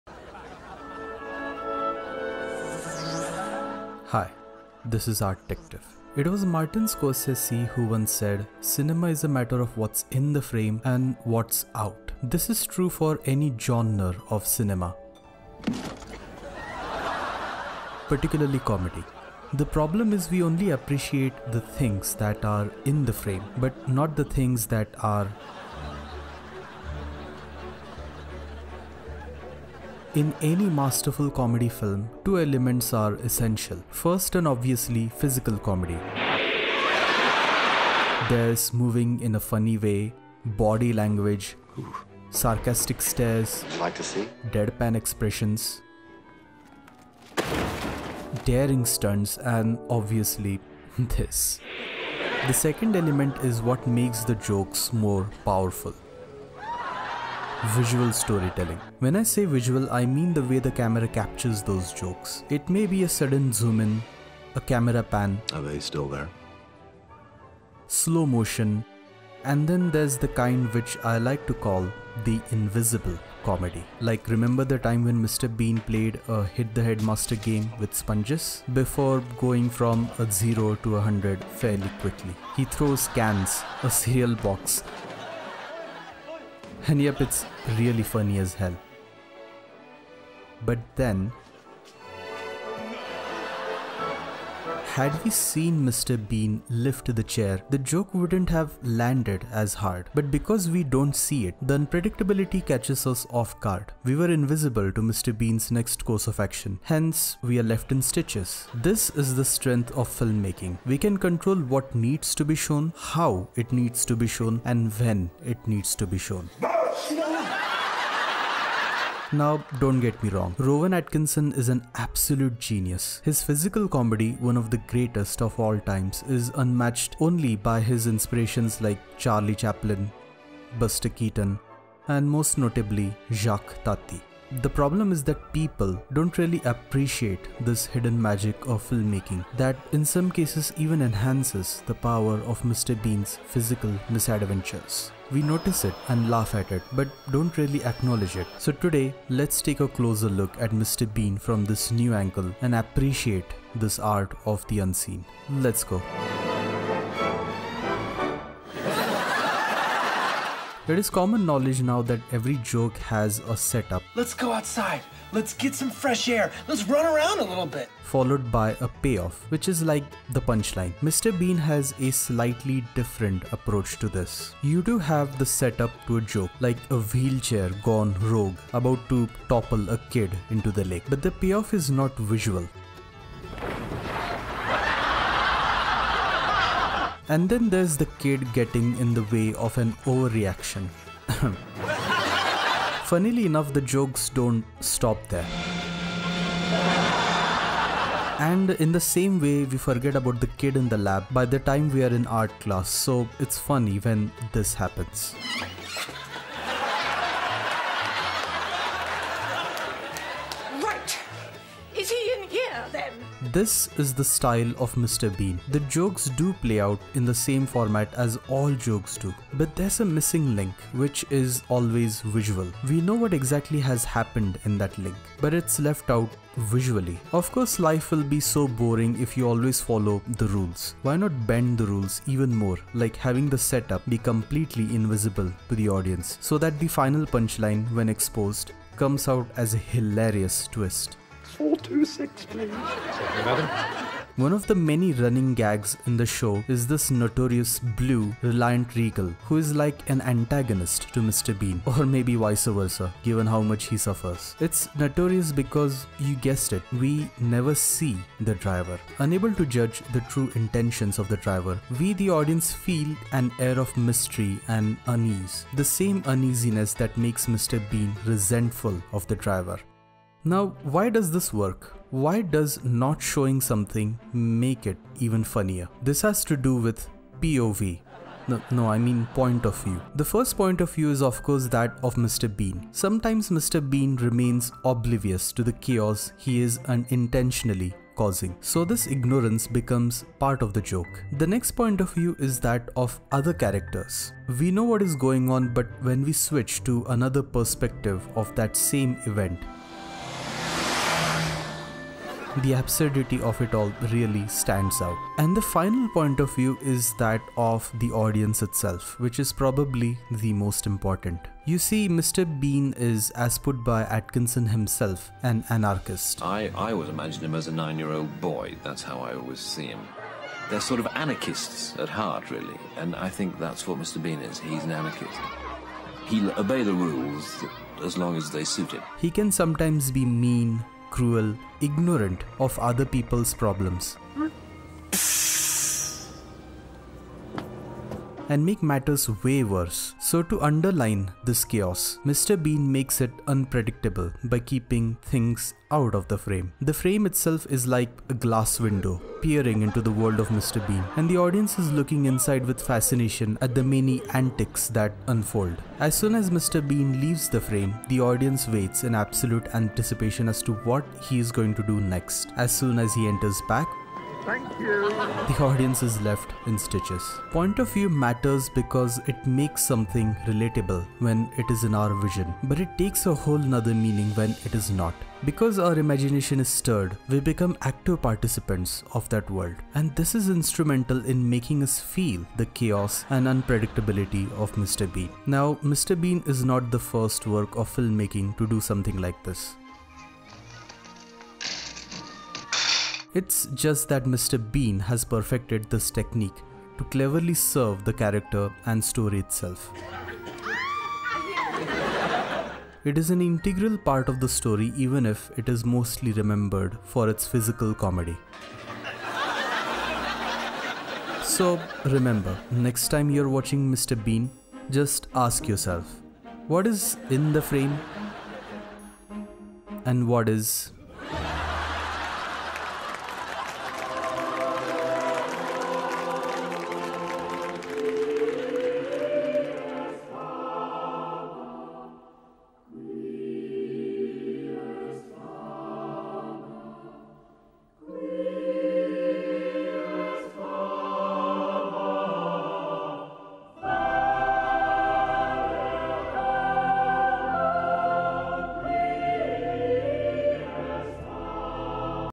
What you don't see in Mr. Bean
Mr Bean still remains one of the greatest feats of physical comedy. But there are still some facets to it that still remain hidden and not really that praised. With it's inspirations like Charlie Chaplin, Buster Keaton and Jacques Tati, it still has so much more to offer. Today's video essay analyzes the invisible comedy of Mr Bean through it's film making, editing and shot composition.

Comment what you want the next video essays to be on!

Arttective is a video essay channel that observes closely the world of art through curious and inquisitive eyes.